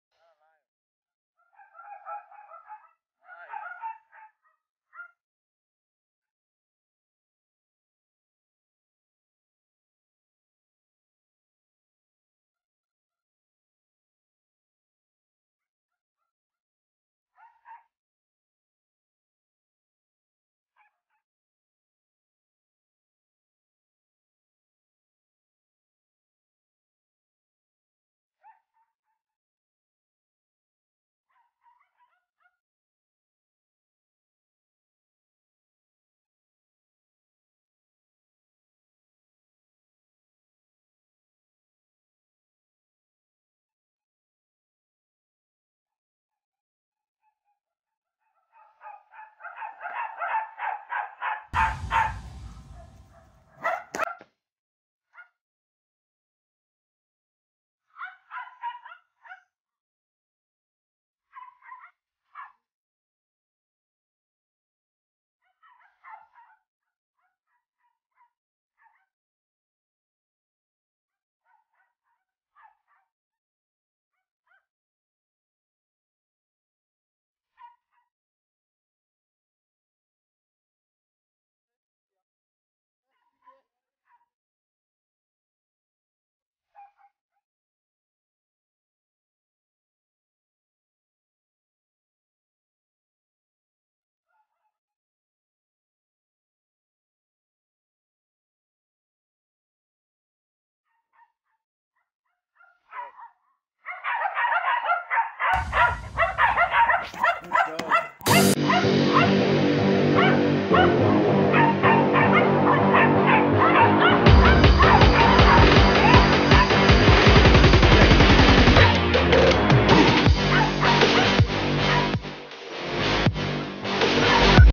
Guard dog pins intruder down#25PAWZPROTECTIONDOGS
Protectiotraining a guard dog to tackle an intruders and pin him down.Their presence discourages intruders.

In the event of a home intrusion, a guard dog will keep the intruders at bay until law enforcement arrives.

They make excellent companions.

Guard dogs are safe to own for families.

They can go with you nearly anywhere.

These dogs provide their owners with extra peace of mind.